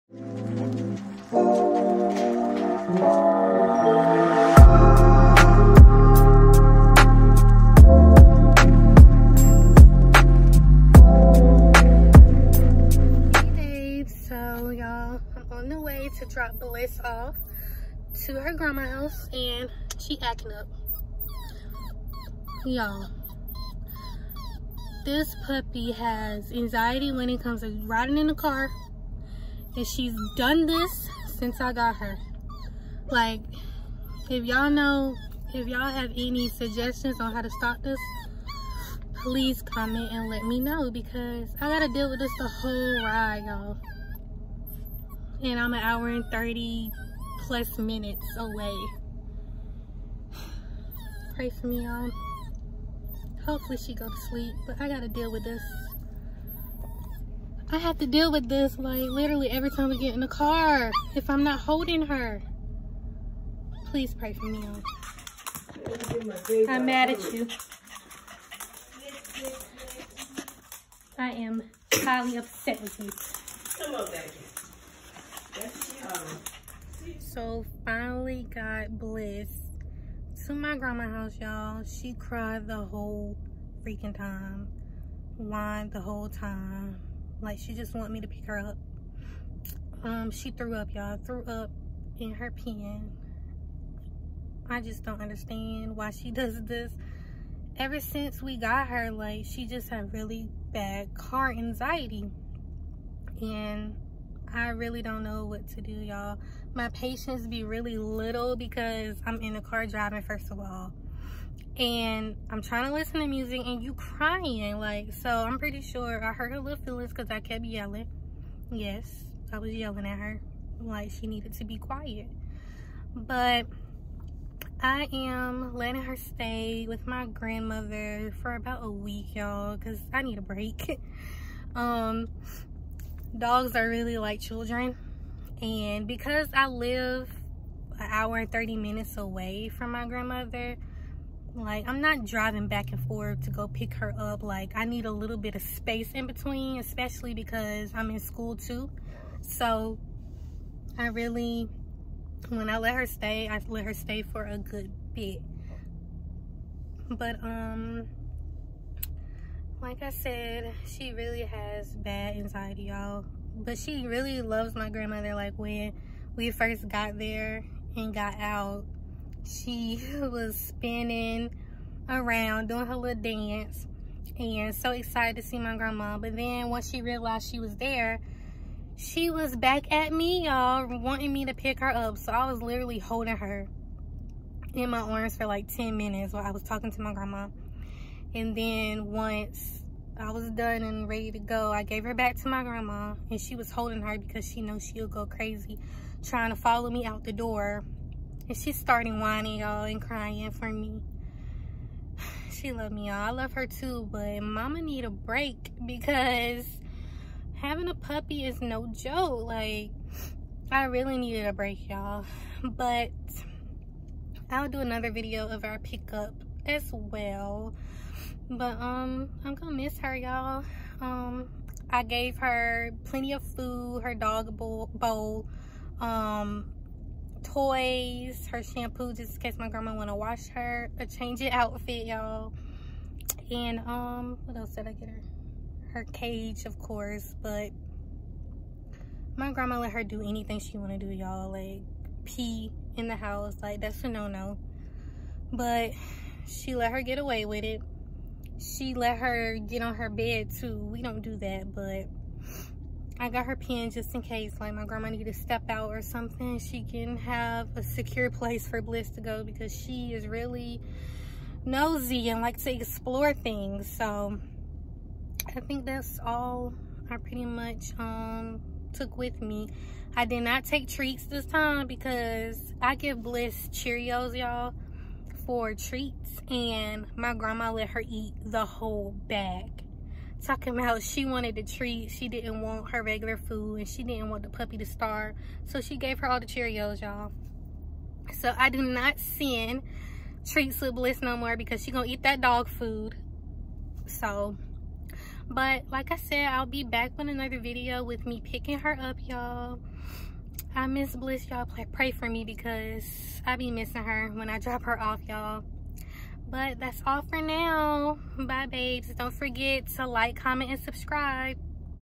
My Toy Poodle Has Car Anxiety!
Come see how Bliss the pup handled her car ride to grandma